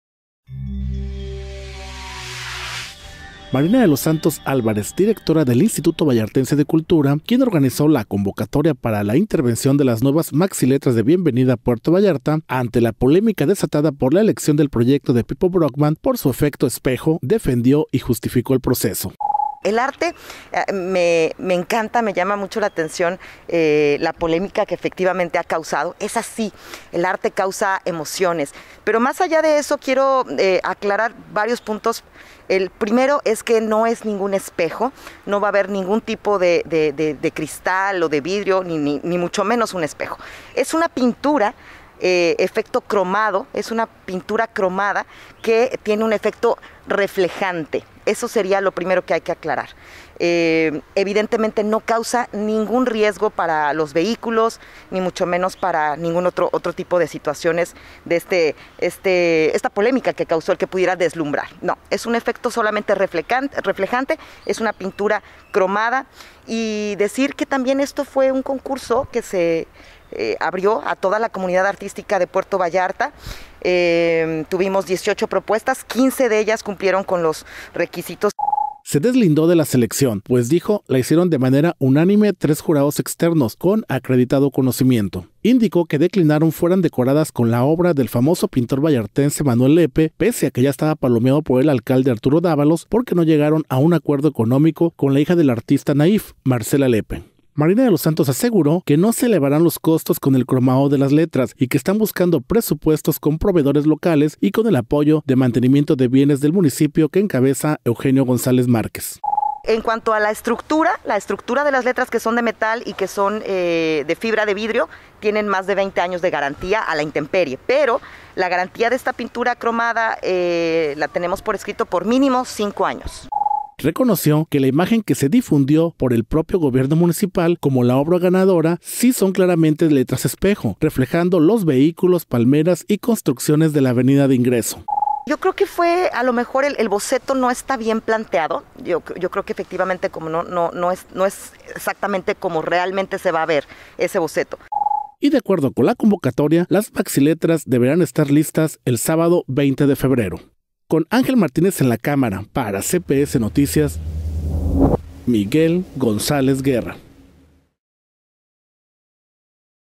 Proyecto de las maxiletras cromadas de Puerto Vallarta, solo tiene garantía para durar cinco años
Reconoce la directora del Instituto Vallartense de Cultura, Marina de los Santos, que el proyecto de las maxiletras cromadas de Puerto Vallarta, solo tiene garantía para durar cinco años.